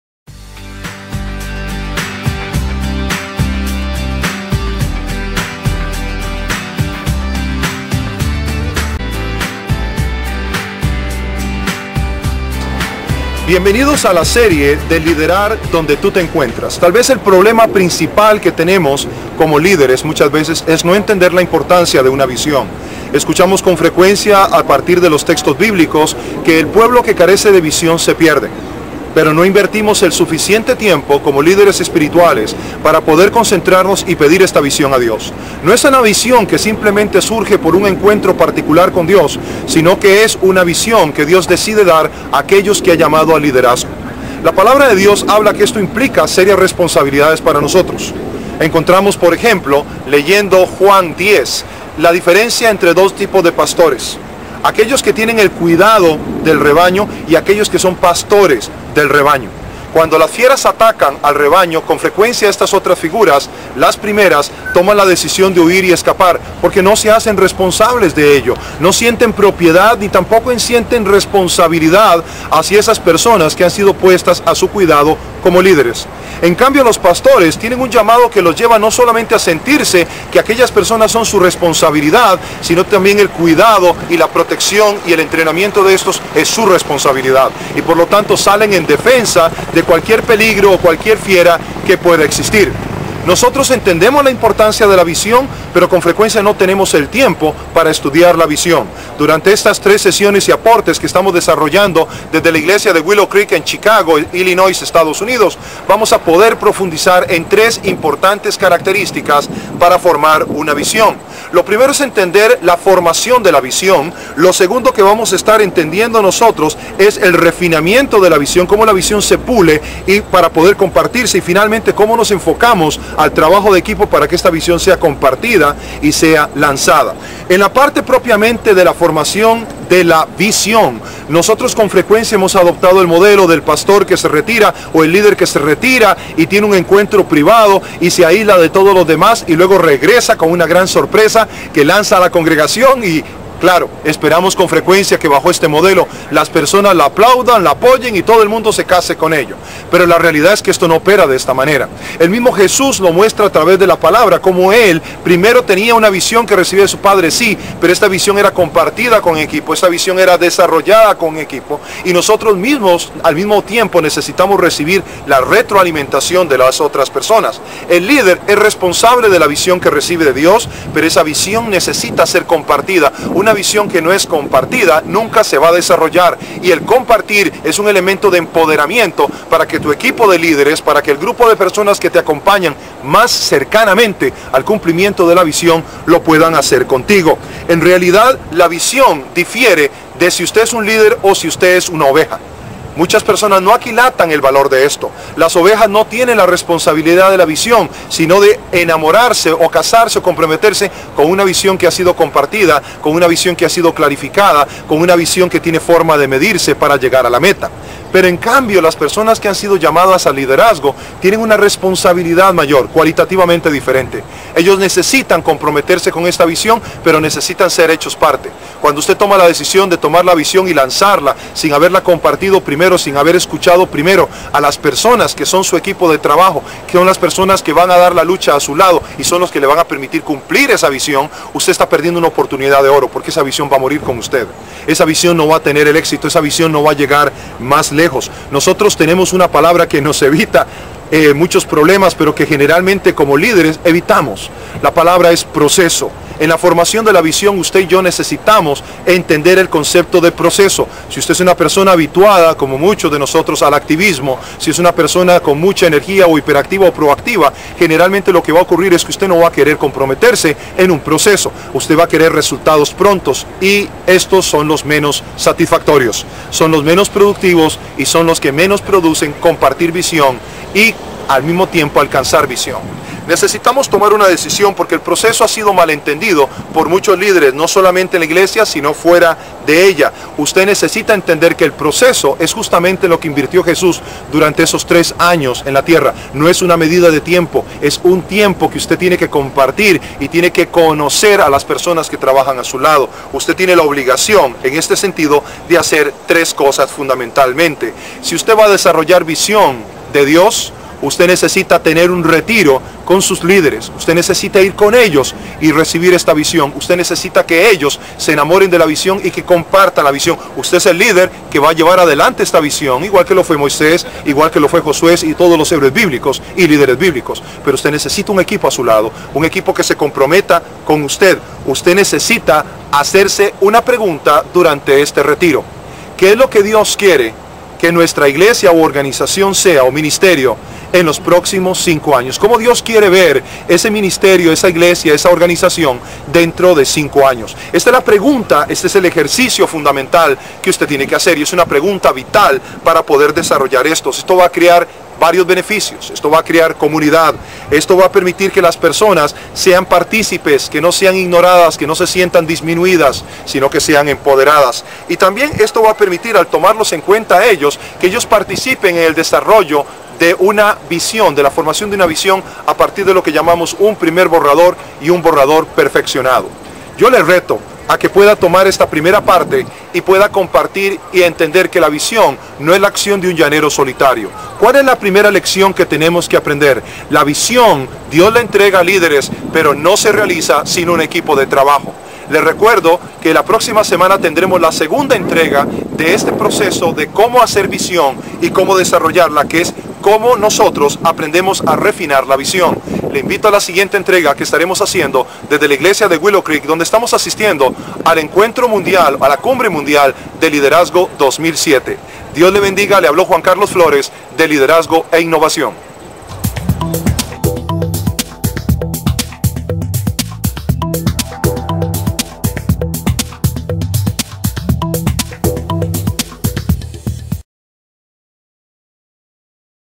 VLOG LiderInnova No 11-07: Visión por la que vale la pena luchar (P.1)
Primera de tres entregas de la serie sobre liderar con una visión por la que vale la pena luchar.
1. FORMACION DE LA VISION: No sea un llanero solitario.
Este episodio del VLOG LiderInnova fue grabado en la Comunidad Cristiana Willow Creek, en South Barrington, Illinois, EUA, en agosto del 2007.